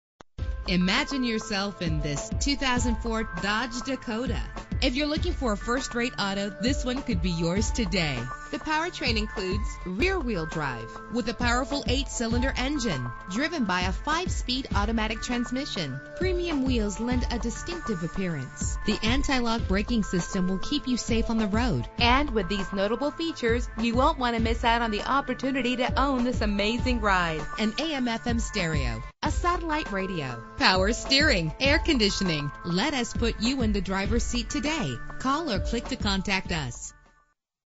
2004 Dodge Dakota - Lawrenceville GA
Phone:
Year: 2004
Make: Dodge
Model: Dakota
Trim: SLT
Engine: 4.7 liter 8 cylinder 16 valve MPFI SOHC
Transmission: 5-Speed Automatic
Color: Patriot Blue Pearlcoat
Mileage: 112069
Address:
719 W. Pike St.